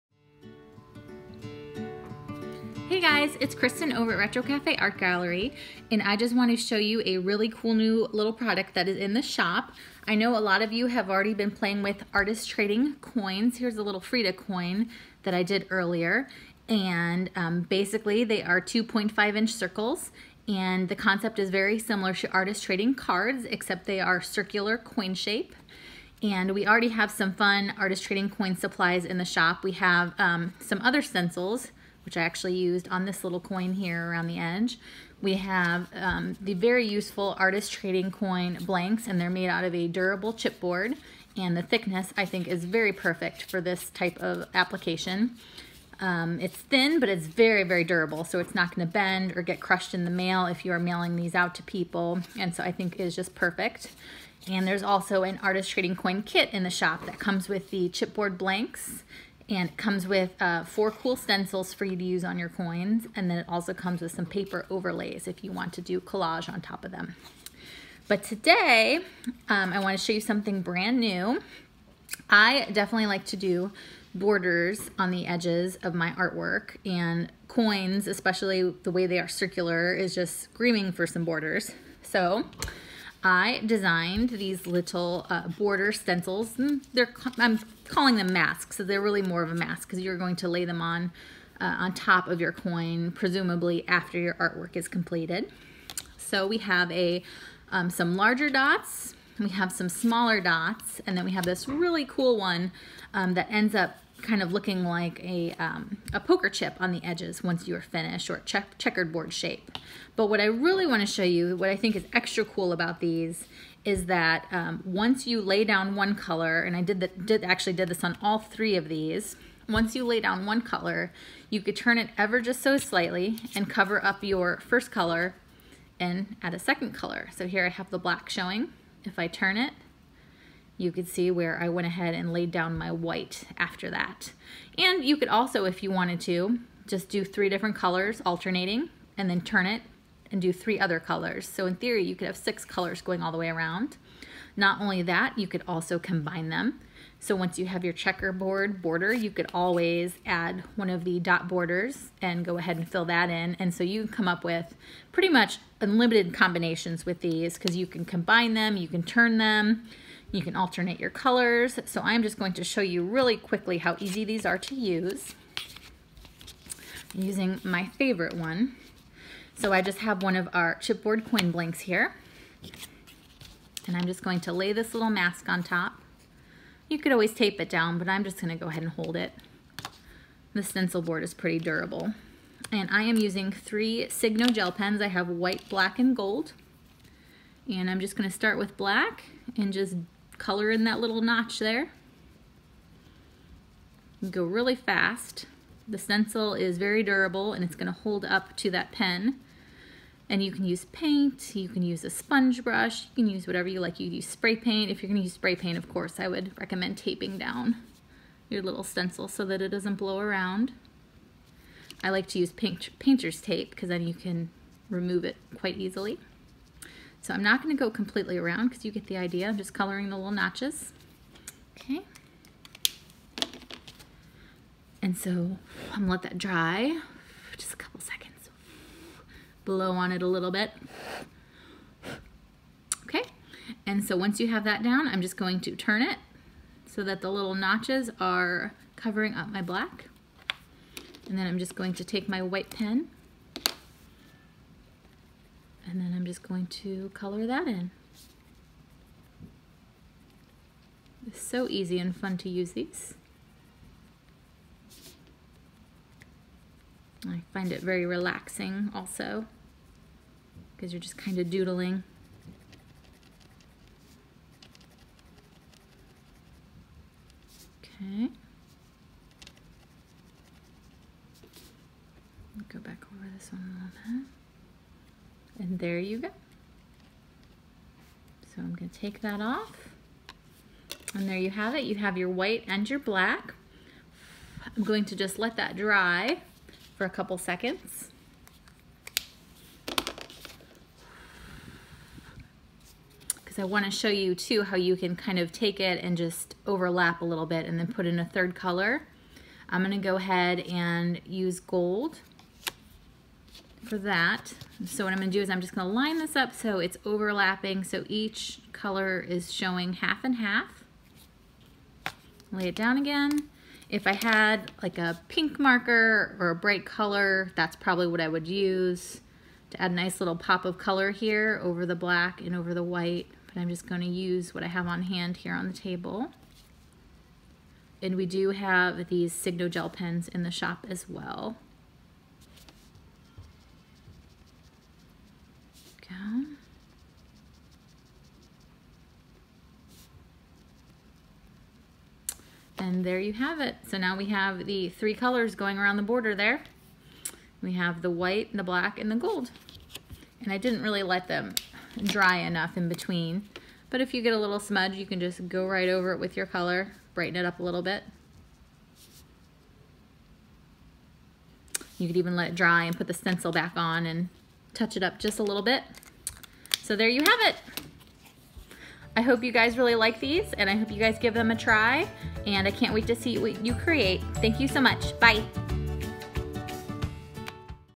Artist Trading Coins Border Mask Stencils In The Shop At Retro Cafe Art Gallery
See how to use our brand new Artist Trading Coin Border Mask Stencils!